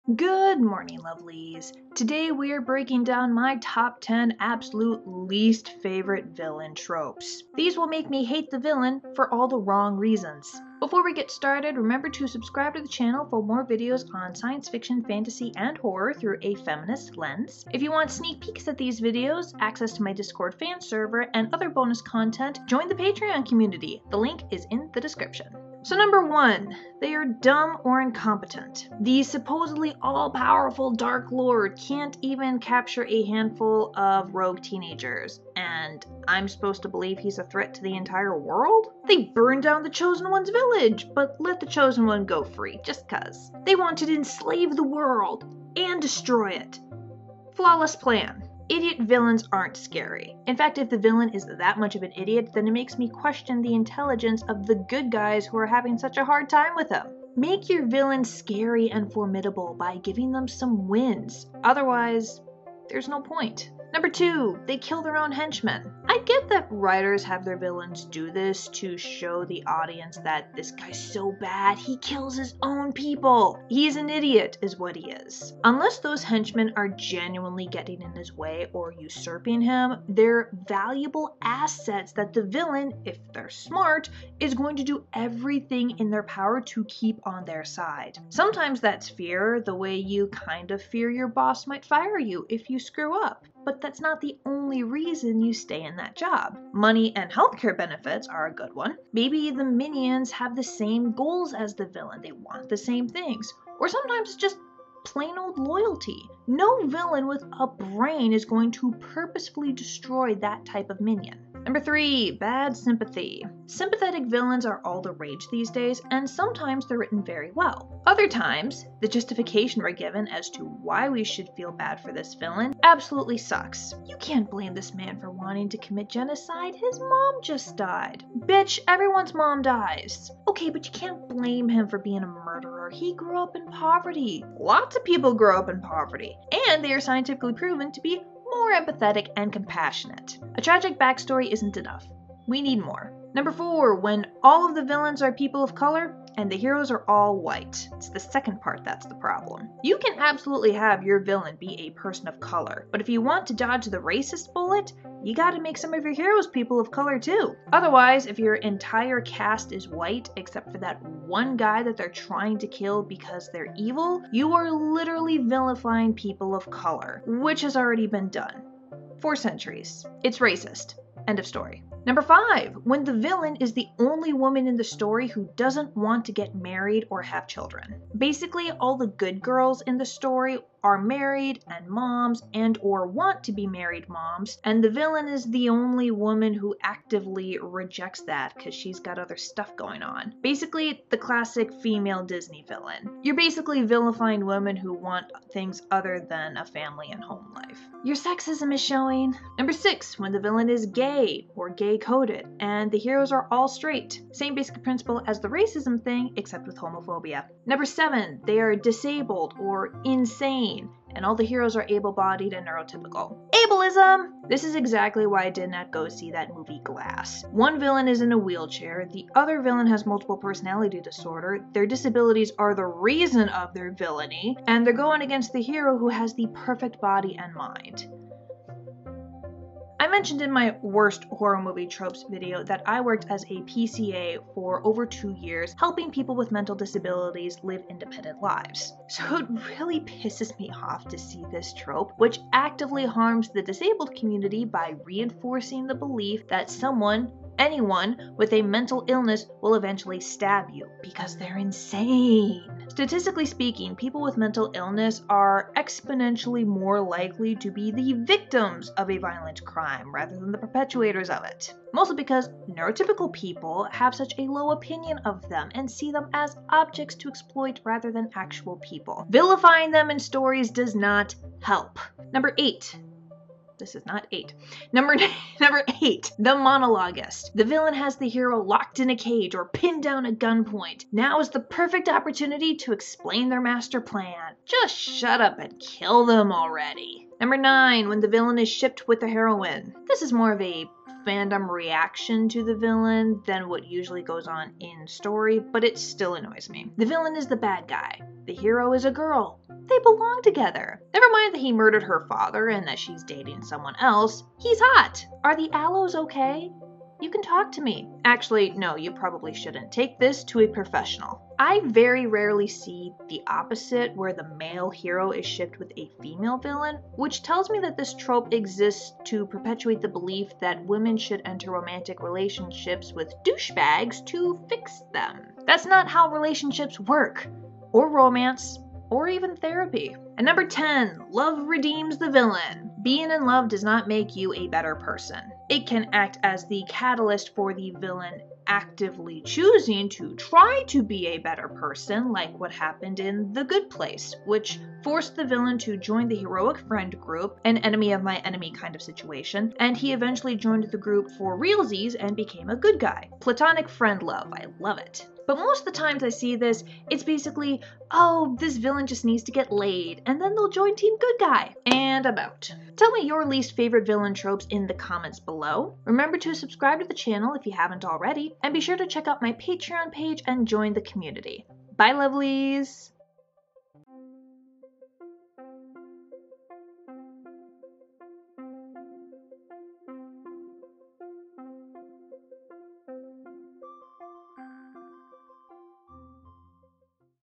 Top 10 WORST Villain Tropes in Science Fiction and Fantasy
My least favorite villain tropes and cliches in sci-fi and fantasy! Dumb villains, "sympathetic" villains with badly written tragic backstories, queercoded villains, monologuing villains, villains shipped with heroines, love redeems the villain...all the bad tropes that are guaranteed to make me hate the antagonist for all the wrong reasons.